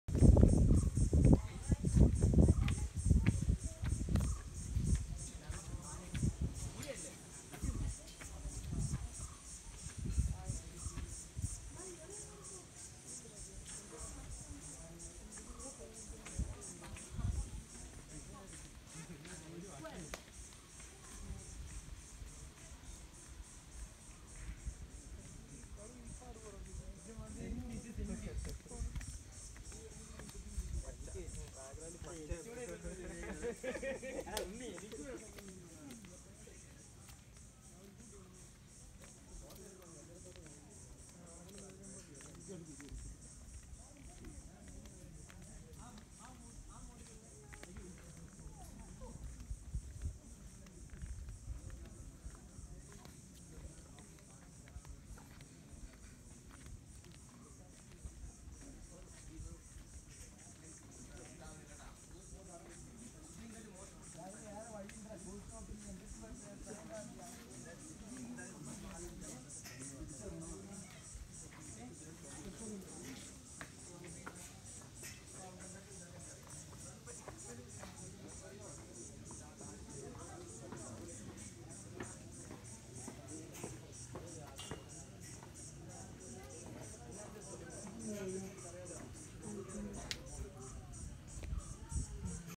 Malayattoor Church [ Ernakulam, Kerala, India ]
The St. Thomas Syro-Malabar Catholic Church, Malayattoor  is one of the most prominent Christian pilgrim centers in the Malayattoor Angamaly, Ernakulam district of Kerala, India. The Malayatoor Church attracts a large number of devotees from not just Kerala, but from all over India.  On the hunters went to the mountain for hunting. While they stayed in the night they saw a glittering sign of cross on the rock. Out of curiosity they struck there with their rude weapons. To their surprise blood gushed out. They ran to the valley and told the locals. They went to the mountain and while they prayed there they got many miracles. This is the humble beginning of Pilgrimage to Kurishumudi. One can easily seek spiritual happiness and satisfaction amidst the calm and serene locales surrounding Malayatoor. In fact, this center is the most popular and largest St. Thomas shrine in India. It is a popular tourist center and many people throng this place to have a look at this magnificent church. Every year on the first Sunday after Easter, thousands of pilgrims throng to this shrine to seek holy blessings. The church has been decorated with beautiful carvings, designs and paintings that depict the life of Jesus Christ.